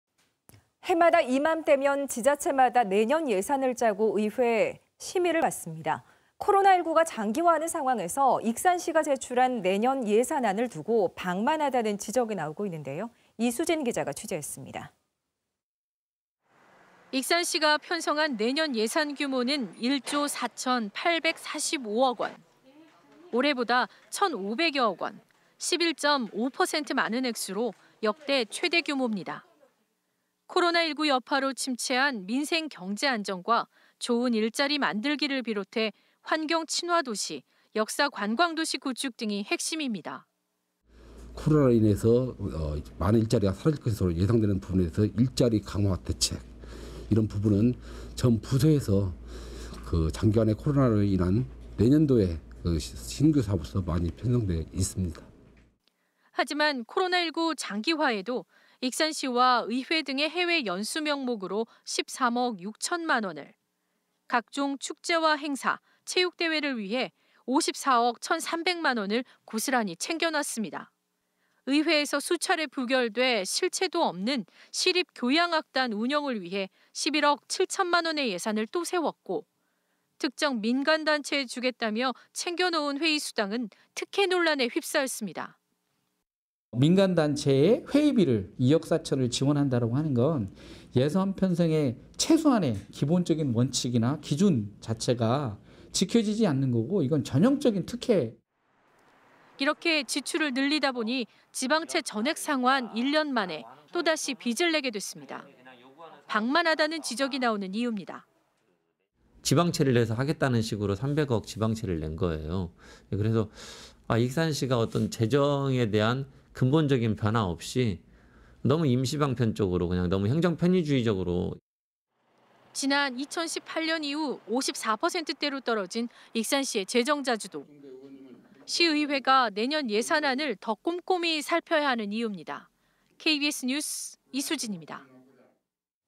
코로나19에도 해외 연수·행사 예산 ‘그대로’
[앵커]

 해마다 이맘때면, 지자체마다 내년 예산을 짜고  의회 심의를 받는데요.

 코로나19가 장기화하는 상황에서 익산시가 제출한 내년 예산안을 두고 방만하다는 지적이 나오고 있습니다.

 이수진 기자가 취재했습니다.

 [리포트]

 익산시가 편성한 내년 예산 규모는 1조 4천8백45억 원.

 올해보다 천5백여억 원,  11.5% 많은 액수로 역대 최대 규모입니다.

 코로나19 여파로 침체한  민생 경제 안정과 좋은 일자리 만들기를 비롯해 환경 친화 도시, 역사관광 도시 구축 등이 핵심입니다.

 [모순영/익산시 기획예산과장 : "코로나로 인해서 많은 일자리가 사라질 것이 예상되는 부분에서 일자리 강화 대책, 이런 부분은 전 부서에서 장기간의 코로나로 인한 내년도 신규사업으로 많이 편성돼 있습니다."]

 하지만 코로나19 장기화에도 익산시와 의회 등의 해외 연수 명목으로 13억 6천만 원을, 각종 축제와 행사, 체육대회를 위해  54억 천3백만 원을 고스란히 챙겨 놨습니다.

 의회에서 수차례 부결돼 실체도 없는 시립교향악단 운영을 위해 11억 7천만 원의 예산을 또 세웠고, 특정 민간단체에 주겠다며 챙겨놓은 회의 수당은  특혜 논란에 휩싸였습니다.

 [이상민/익산참여연대 사무처장 : "민간단체의 회의비를 2억 4천을 지원한다고 하는 건 예산 편성의 최소한의 기본 원칙이나 기준 자체가 지켜지지 않는 거고 이건 전형적인 특혜…."]

 이렇게 지출을 늘리다 보니, 지방채 전액 상환 1년 만에 또다시 빚을 내게 됐습니다.

 방만하다는 지적이 나오는 이유입니다.

 [임형택/익산시의원 : "지방채를 내서 하겠다는 식으로 3백억 지방채를 낸 거예요. 익산시가 재정의 근본적인 변화 없이 너무 임시방편으로 행정 편의주의적으로…."]

 지난 2018년 이후 54 퍼센트 대로 떨어진 익산시의 재정자주도·시의회가 내년 예산안을 더 꼼꼼히 살펴야 하는 이유입니다.

 KBS 뉴스 이수진입니다.